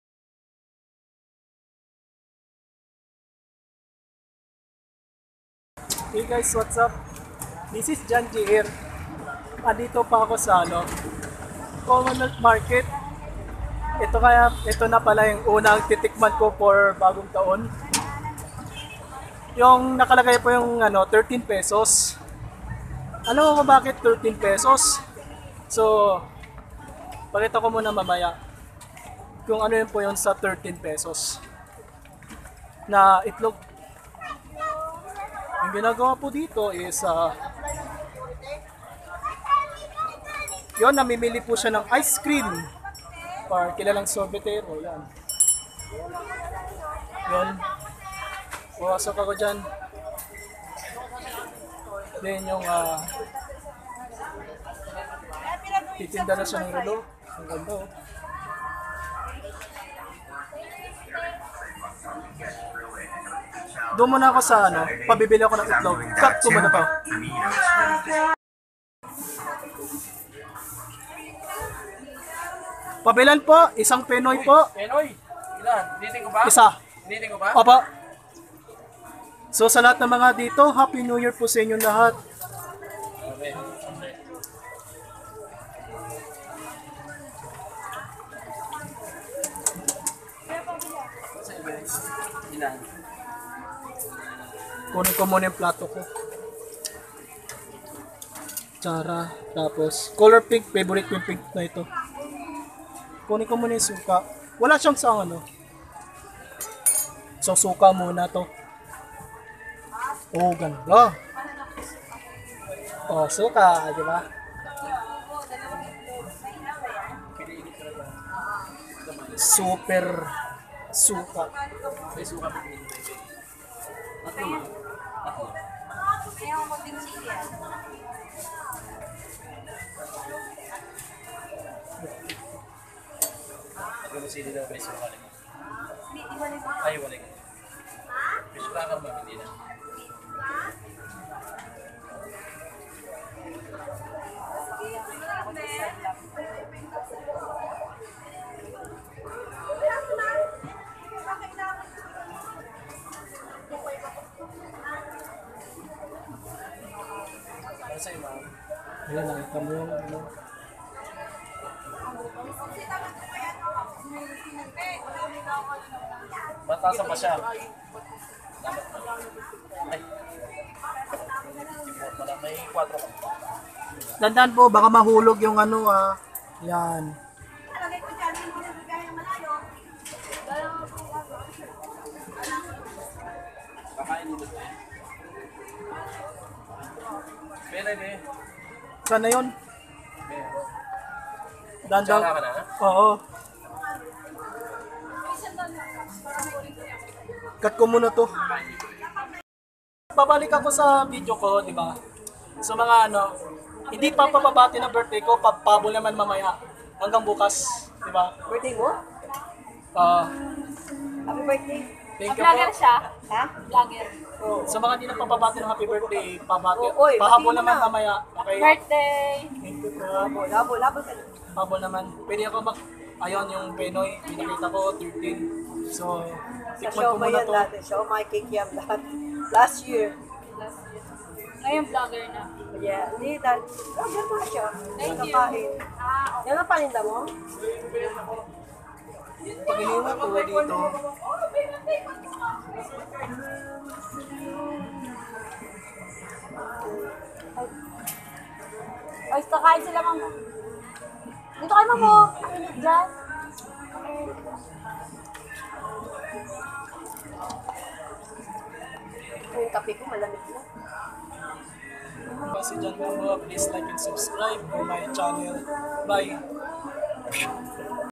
JohnGEats | Season 2 | Episode #25 ft. "PENOY" with Vinegar
Starting Kick off with the Bang
Featuring the "PENOY" with Vinegar
And also Happy Birthday to Myself
"JOHN L. GAMBOA"